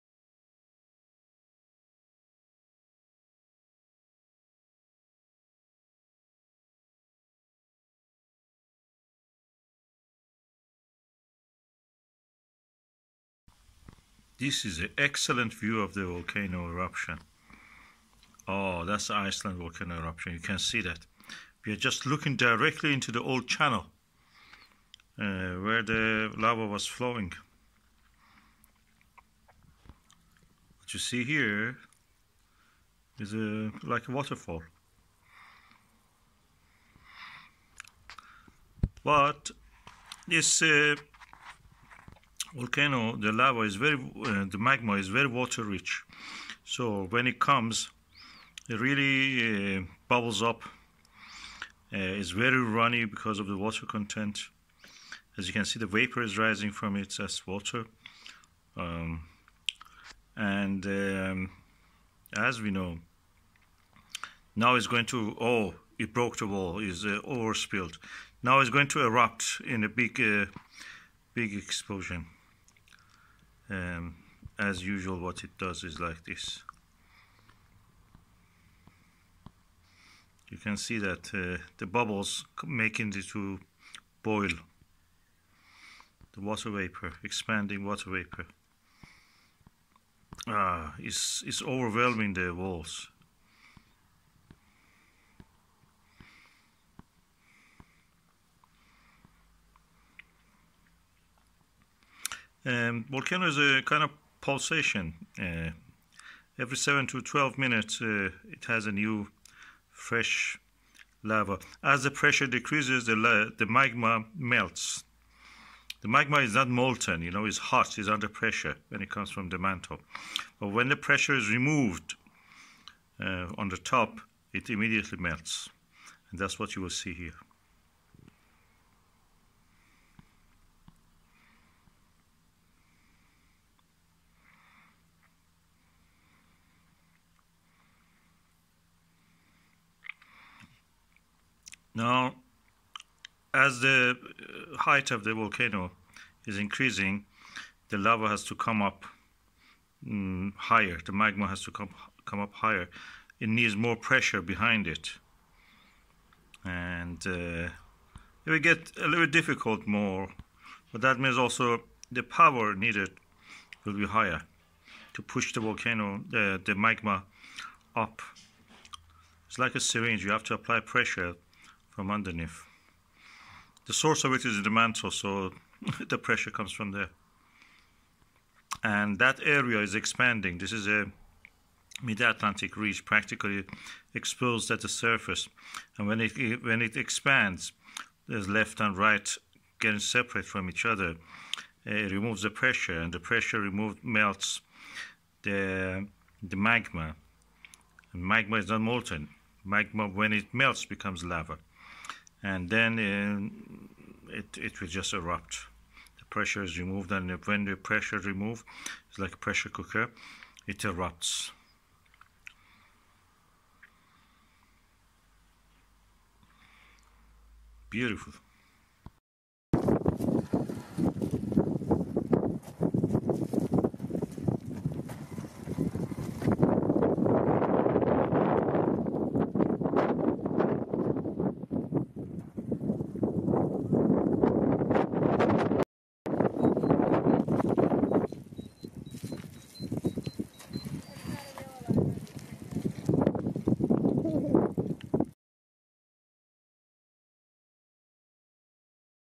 Water Rich Magma Erupts In Iceland Fagradalsfjall Volcano
Volcano Opening New Channels for Lava Flow.

7th June : Volcano Wall Breaking Apart.

URGENT: Atlantic Ocean Splitting at the Iceland Fagradalsfjall Volcano

6the June 2021 : World Internet In Danger by Iceland Volcano.

5 June 2021: The Western Wall or Second Wall is now flooded.

On 19 March 2021, an effusive eruption started at approximately 8:45 PM local time in Geldingadalir ([ˈcɛltiŋkaˌtaːlɪr̥] "gelding valleys", the singular "Geldingadalur" [-lʏr̥] is also often used) to the south of Fagradalsfjall, the first known eruption on the peninsula in about 800 years. Fagradalsfjall has been dormant for 6,000 years. The eruptive activity was first announced by the Icelandic Meteorological Office at 9:40 PM.[23] Reports state a 600–700-metre-long (2,000–2,300 ft) fissure vent began ejecting lava,[24] which covered an area of less than 1 square kilometre (0.39 sq mi). Currently, the lava flows pose no threat to residents, as the area is mostly uninhabited, although there is potential for sulfur dioxide pollution.

The eruption pattern changed on 2 May from a continuous eruption and lava flow to a pulsating one, where periods of eruptions alternate with periods of inactivity. The magma jets became stronger, producing lava fountains of 300 m (980 ft) in height, visible from Reykjavík, with the highest one measured at 460 m (1,510 ft). The lava flow rate in the following weeks was also double that of the average for the first six weeks.